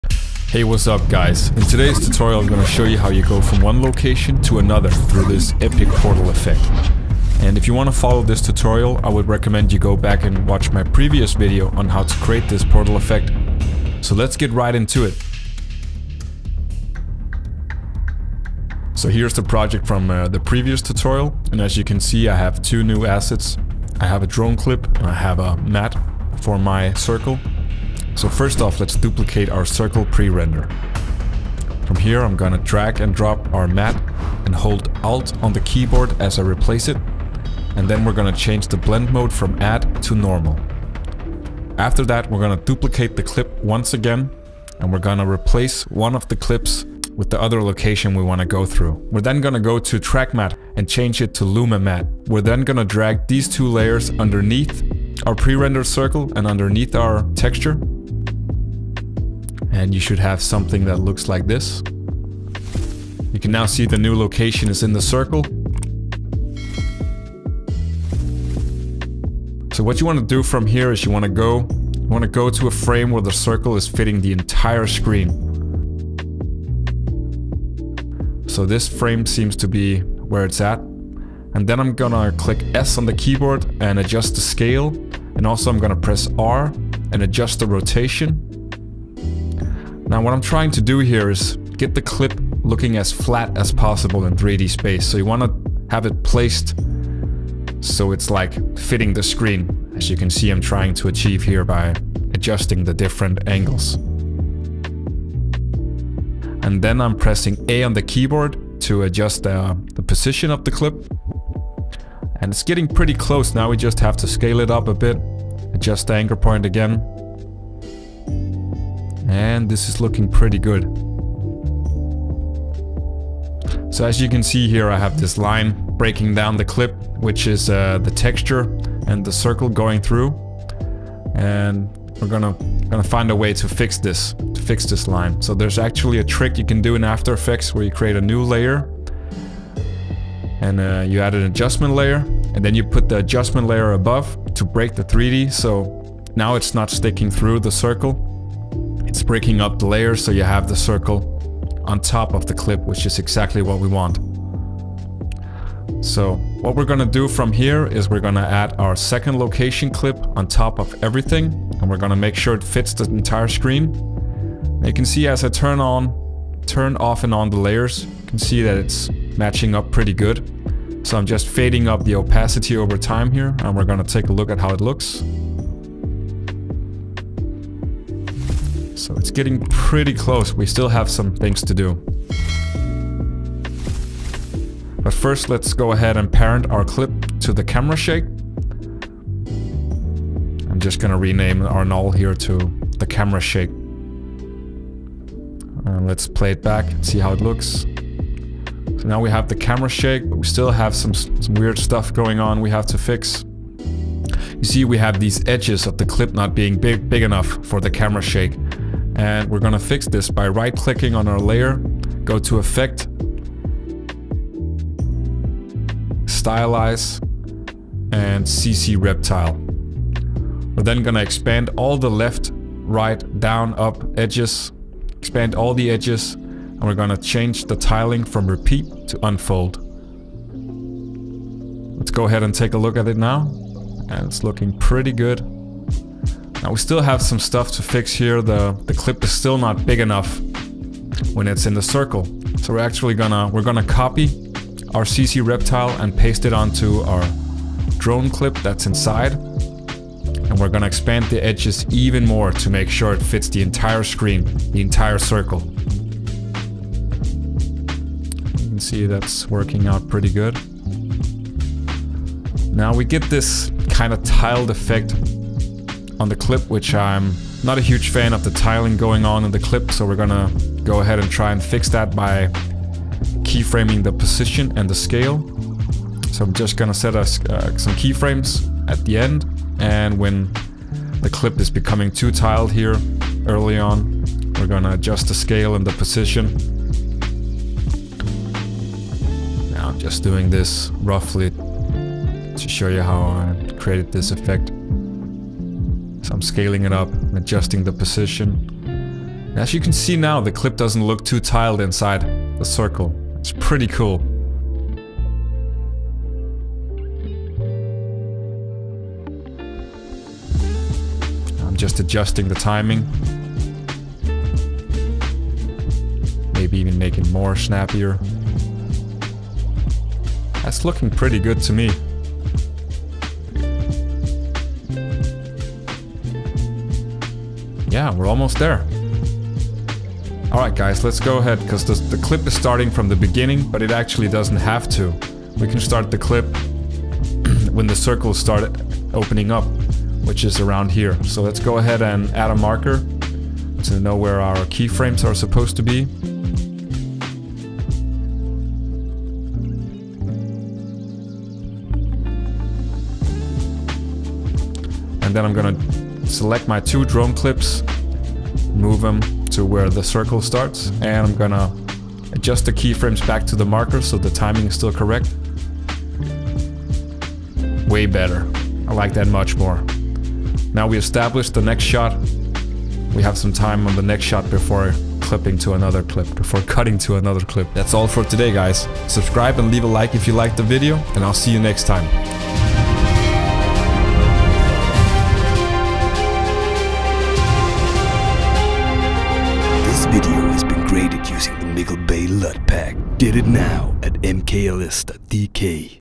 After Effects Tutorial How To Portal Transition From One Location To Another
Today's Video: How To Portal Transition From One Location To Another - After Effects Tutorial 2022